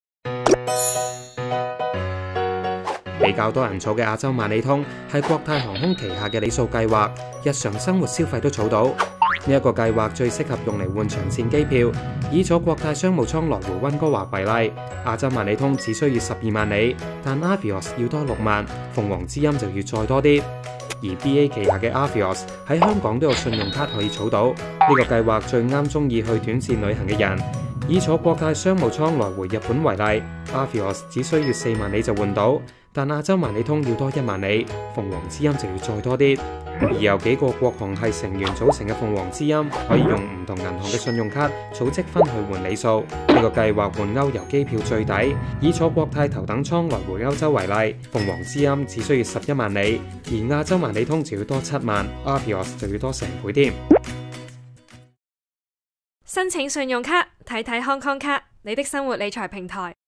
Asia Miles X Avios X 鳳凰知音 三大里數計劃比較
咁多信用卡儲唔同計劃花多眼亂
其實識得揀里數計劃都好重要
今次就同大家分享下
等你快啲換到機票啦！